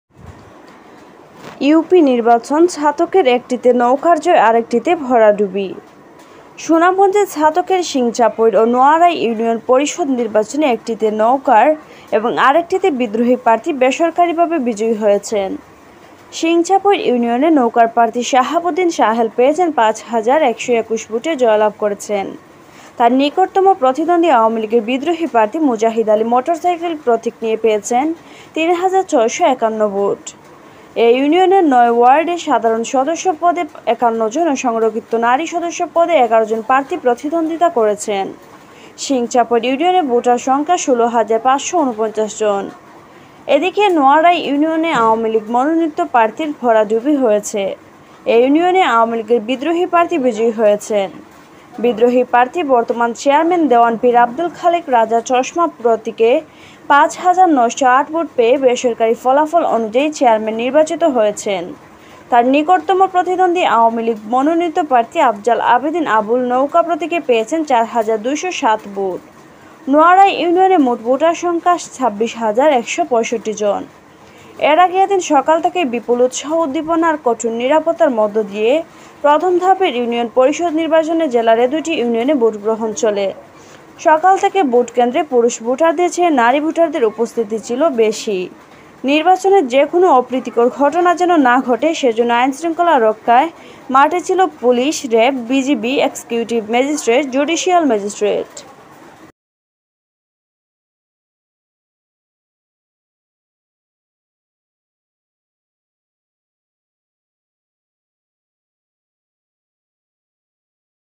ইউপি নির্বাচন: ছাতকের একটিতে নৌকার জয়, আরেকটিতে ভরাডুবি
সুনামগঞ্জ প্রতিনিধি
প্রকাশিত : ২১ জুন, ২০২১ ২০:১২

সুনামগঞ্জ প্রতিনিধি :: সুনামগঞ্জের ছাতকের সিংচাপইড় ও নোয়ারাই ইউনিয়ন পরিষদ নির্বাচনে একটিতে নৌকার এবং আরেকটিতে বিদ্রোহী প্রার্থী বেসরকারিভাবে বিজয়ী হয়েছেন।

সিংচাপইড় ইউনিয়নে নৌকার প্রার্থী সাহাব উদ্দিন সাহেল পেয়েছেন ৫ হাজার ১২১ ভোটে জয়লাভ করেছেন। তার নিকটতম প্রতিদ্বন্দ্বী আওয়ামী লীগের বিদ্রোহী প্রার্থী মোজাহিদ আলী (মোটরসাইকেল) প্রতিক নিয়ে পেয়েছেন ৩ হাজার ৬৫১ ভোট।

এ ইউনিয়নের ৯ ওয়ার্ডে সাধারণ সদস্য পদে ৫১ জন ও সংরক্ষিত নারী সদস্য পদে ১১ জন প্রার্থী প্রতিদ্বন্দ্বিতা করেছেন। সিংচাপইড় ইউনিয়নে ভোটার সংখ্যা ১৬ হাজার ৫৪৯ জন।

এদিকে, নোয়ারাই ইউনিয়নে আওয়ামীলীগ মনোনীত প্রার্থীর ভরাডুবি হয়েছে। এ ইউনিয়নে আওয়ামীলীগের বিদ্রোহী প্রার্থী বিজয়ী হয়েছেন। বিদ্রোহী প্রার্থী বর্তমান চেয়ারম্যান দেওয়ান পীর আব্দুল খালিক রাজা চশমা প্রতীকে ৫ হাজার ৯০৮ ভোট পেয়ে বেসরকারি ফলাফল অনুযায়ী চেয়ারম্যান নির্বাচিত হয়েছেন। তার নিকটতম প্রতিদ্বন্দ্বী আওয়ামীলীগ মনোনীত প্রার্থী আফজাল আবেদীন আবুল নৌকা প্রতীকে পেয়েছেন ৪ হাজার ২০৭ ভোট।

নোয়ারাই ইউনিয়নে মোট ভোটার সংখ্যা ২৬ হাজার ১৬৫ জন।

এর আগে, এদিন সকাল থেকে বিপুল উৎসাহ উদ্দীপনা আর কঠোর নিরাপত্তার মধ্যে দিয়ে প্রথম ধাপের ইউনিয়ন পরিষদ নির্বাচনে জেলার এ দুটি ইউনিয়নে ভোটগ্রহণ চলে।

সকাল থেকে ভোট কেন্দ্রে পুরুষ ভোটারদের চেয়ে নারী ভোটারদের উপস্থিতি ছিলো বেশী।

নির্বাচনে যেকোনো অপ্রীতিকর ঘটনা যেন না ঘটে সেজন্য আইনশৃঙ্খলা রক্ষায় মাটে ছিলো পুলিশ, র‍্যাব, বিজিবি, এক্সিকিউটিভ ম্যাজিস্ট্রেট, জুডিশিয়াল ম্যাজিস্ট্রেট